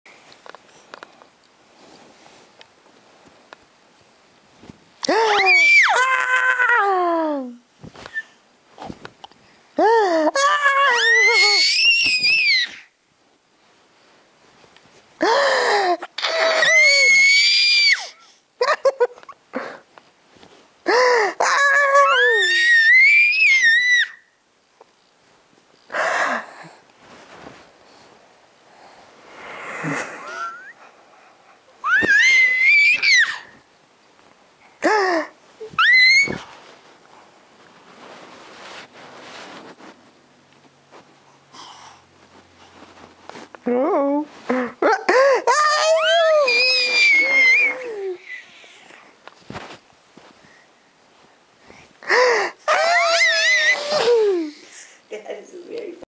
Hiding under the covers
Im hiding under the covers... lets see if Emma can find me!